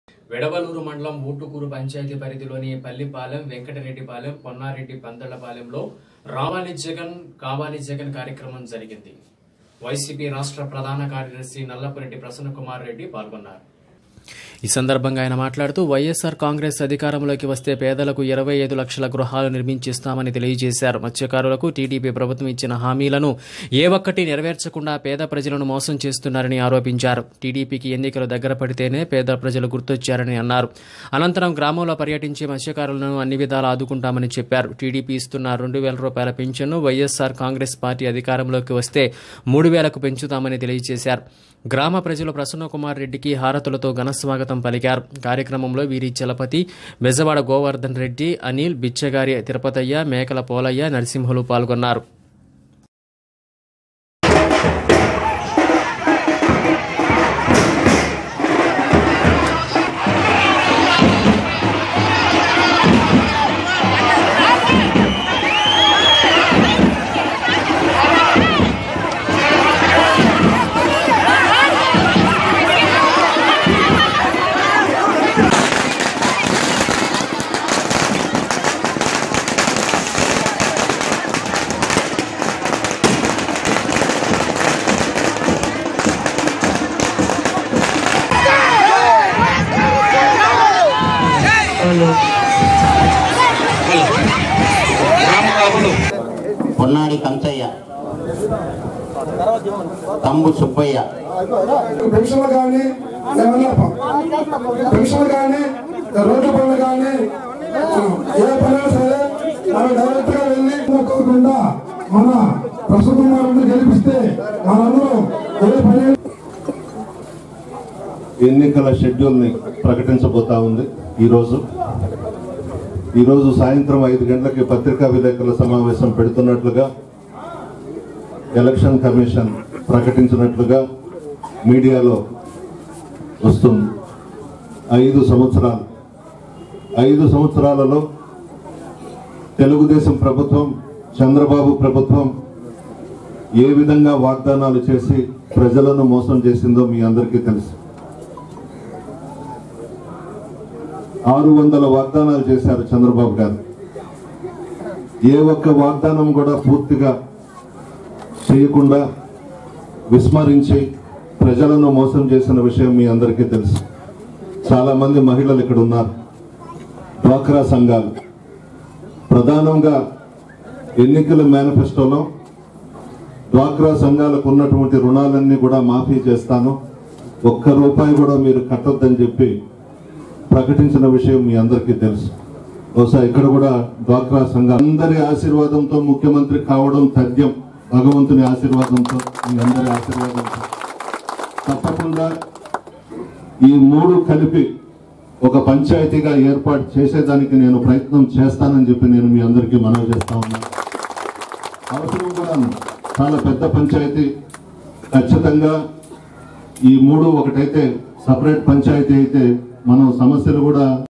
vidavaluru: nallapureddy prasanna kumar reddy "ravali jagan kavali jagan" || Dn5
విడవలూరు మండలం ఊటుకూరు పంచాయతీ పరిధిలోని పల్లిపాలెం, వెంకటరెడ్డి పాలెం, పొన్నపూడి పెద్దపాళెంలో  రావాలి జగన్ కావాలి జగన్ కార్యక్రమం జరిగింది. వైసిపి రాష్ట్ర ప్రధాన కార్యదర్శి నల్లపురెడ్డి ప్రసన్నకుమార్ రెడ్డి  పాల్గొన్నారు.
ఈ సందర్భంగా ఆయన మాట్లాడుతూ వైఎస్ఆర్ కాంగ్రెస్ అధికారంలోకి వస్తే పేదలకు 25లక్షల గృహాలు నిర్మించి  ఇస్తామని తెలిపారు. మత్స్యకారులకు టిడిపి ప్రభుత్వం ఇచ్చిన హామీలను ఏ ఒక్కటి నెరవేర్చ కుండా పేద ప్రజలను మోసం చేస్తున్నారని ఆరోపించారు. టిడిపికి ఎన్నికలు దగ్గరపడితేనే పేద ప్రజలు గుర్తుకొచ్చారని అన్నారు.  అనంతరం గ్రామంలో పర్యటించి మత్స్యకారులను అన్ని విధాలా ఆదుకుంటామని చెప్పారు. టిడిపి ఇస్తున్న 2 వేల పింఛన్ను వైస్సార్ కాంగ్రెస్ పార్టీ అధికారంలోకి వస్తే 3 వేలకు పెంచుతామని తెలిపారు. గ్రామ ప్రజలు ప్రసన్న కుమార్ రెడ్డికి హారతులతో ఘన స్వాగతం పలికారు. కార్యక్రమంలో వీరి చలపతి, బెజవాడ గోవర్ధన్ రెడ్డి, అనిల్, బిచ్చగారి తిరపతయ్య, మేకల పోలయ్య, నరసింహులు పాల్గొన్నారు.